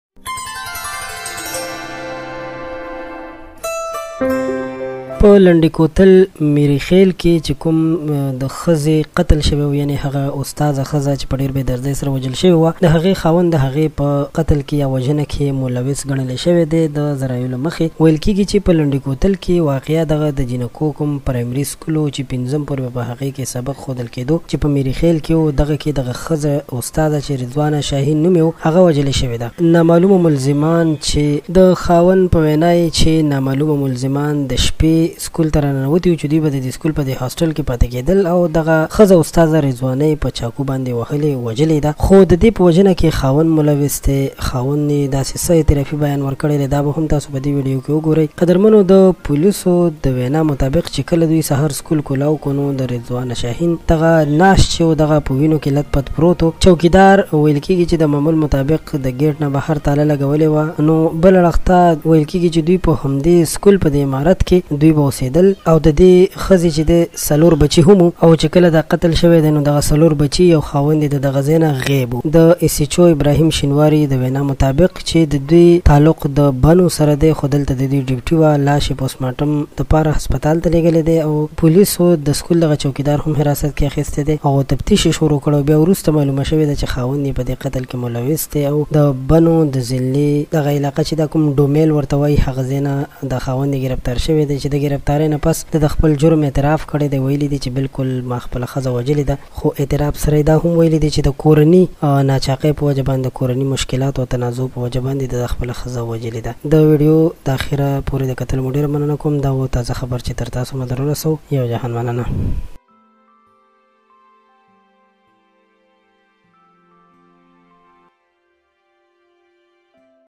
لنډی کوتل کی خاوند خپله خزه داسی ق.تل.کړی اوفف د خاوند یان | Landi kotal waqia
لنډی کوتل کی خاوند خپله خزه داسی ق.تل.کړی اوفف د خاوند یان | Landi kotal waqia yaw khawand khpala khaza owajala. Dagha khaza e education officer hm wa